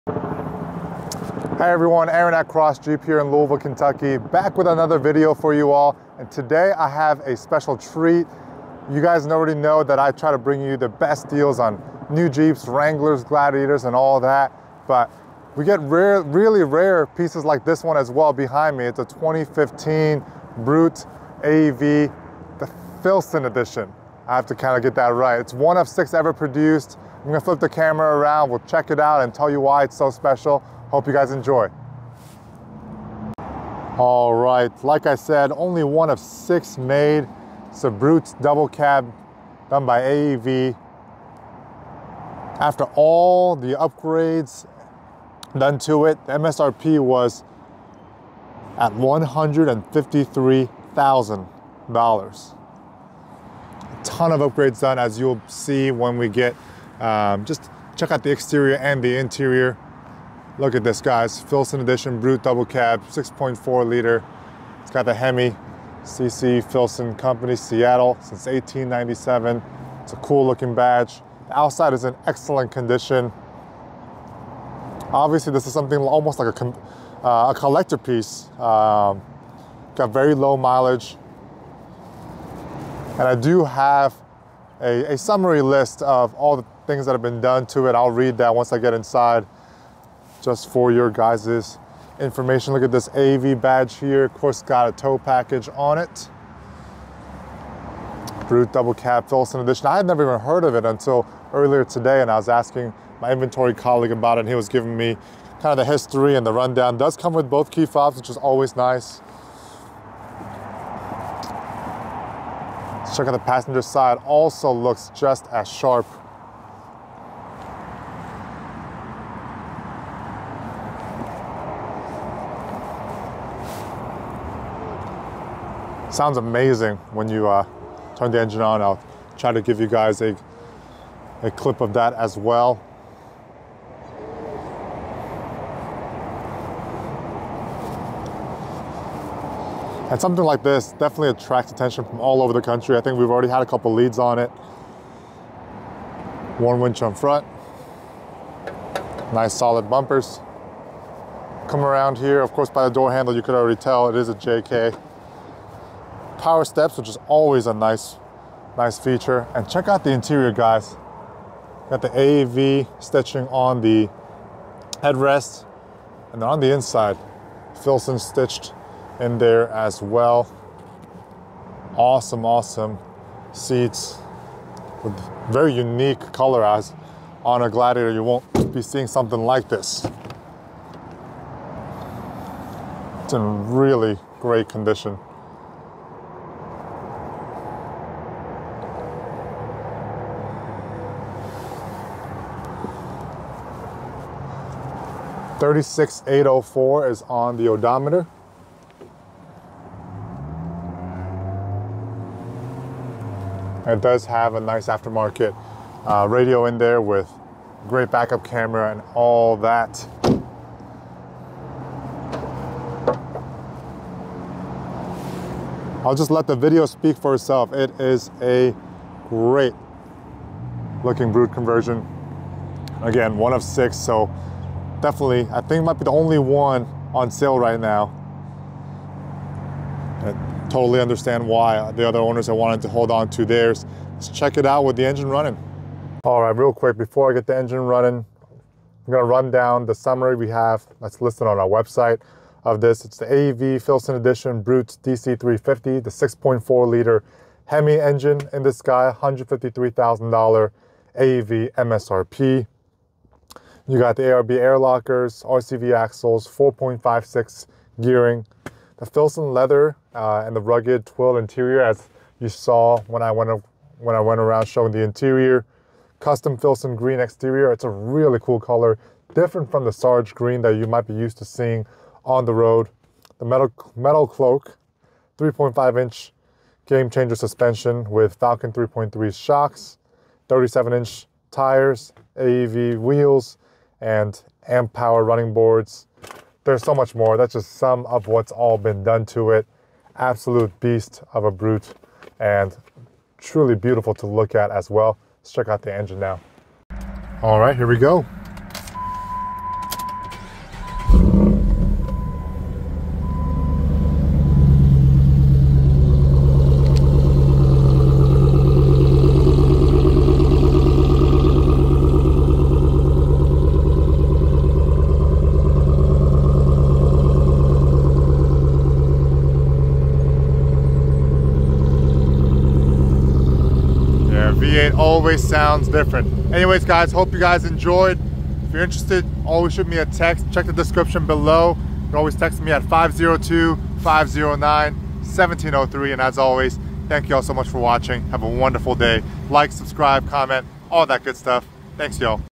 SOLD --- 2015 AEV Filson Edition 6.4L HEMI Brute! 1 of 6 Ever Made! Now ONLY $77,500!
To see what other customers have said about working with me, check out some reviews at aaronatcrossjeep.com

I look forward to serving you :)

AEV Filson Edition Brute DC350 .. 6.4 HEMI .. $153k AEV MSRP .. Dynatrac 60/44 Upgrade with ARB Air Lockers, RCV Axles, and 4.56 Gearing .. FIlson Leather and Rugged Twill Interior .. Custom Filson Green Exterior .. Metalcloak 3.5 Inch Game Changer Suspension with Falcon 3.3 Shocks .. 37 Inch Nitto Tires on AEV Savegre Wheels .. Amp Power Running Boards .. Steer Smarts Steering

Stock: P7137A
Miles: 36,649